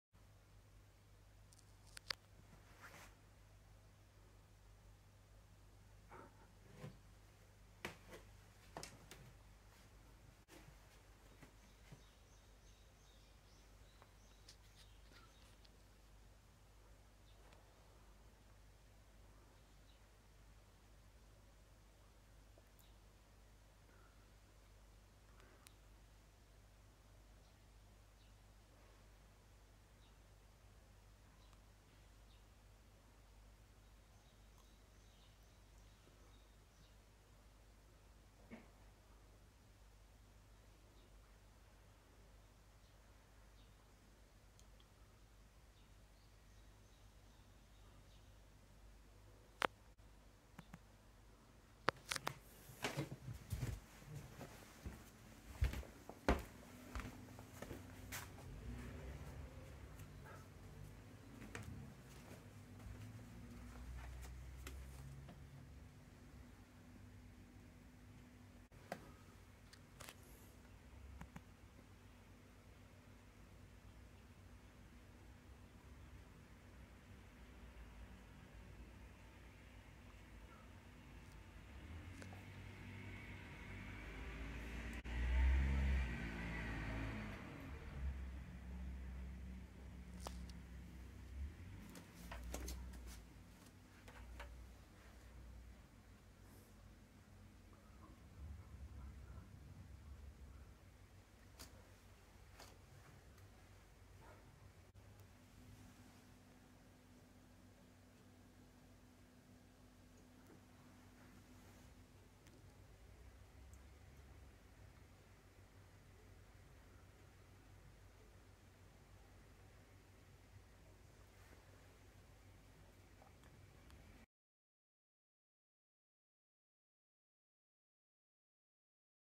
今日は白黒のニューフェイス現る
今日は偶然、白黒君を撮影できました。
それにしても今年はたくさんの猫が自宅周辺に現れています。
〇茶トラ　2月　1回のみ
〇キジトラ　3月　継続中
〇白黒（白多い）　5月　1回のみ
●黒　5月　継続中
●白黒（黒多い）本日　6月

（2022年6月23日午後撮影、pixel4a）